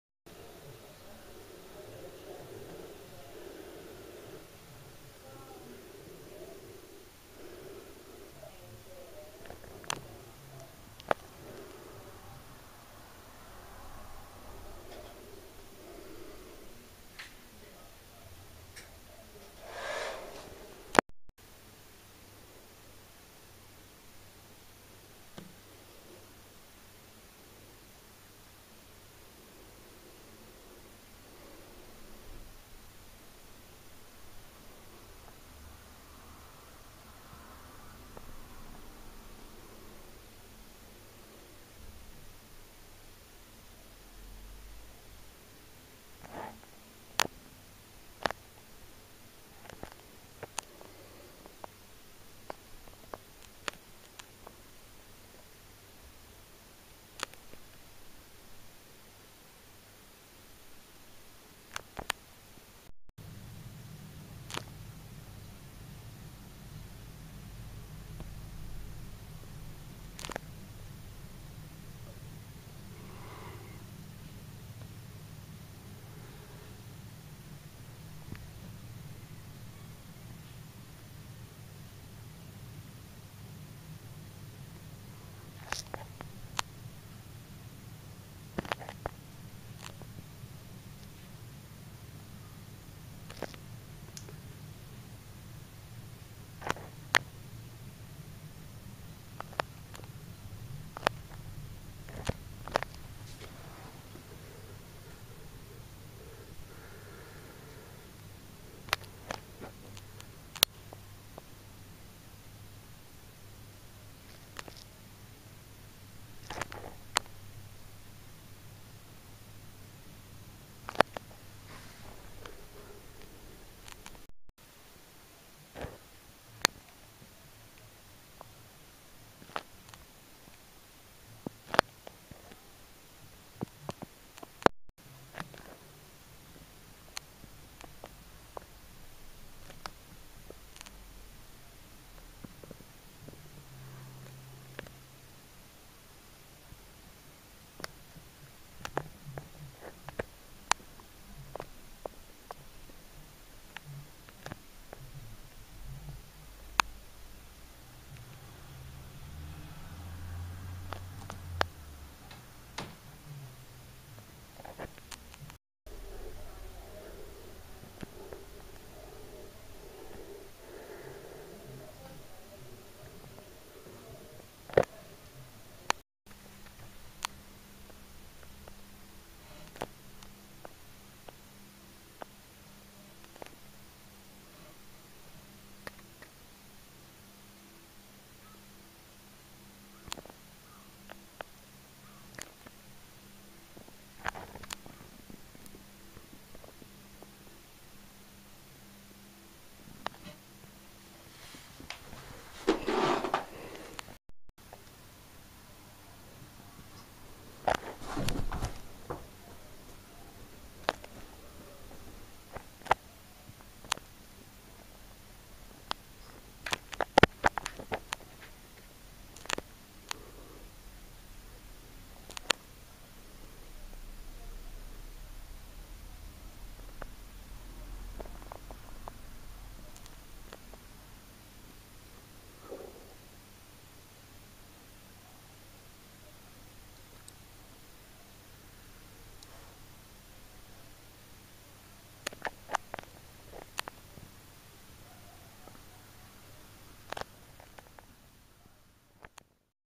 blackbird and tarantula
brachypelma rosea subadult.eyes are bigger than its belly! what was left of the bird was removed after 10 hours in the intrests of health and saftey.
oh and by the way it wasnt my spider that killed the bird it was the murdering sicko cat as usual.my angel wouldnt hurt a fly :)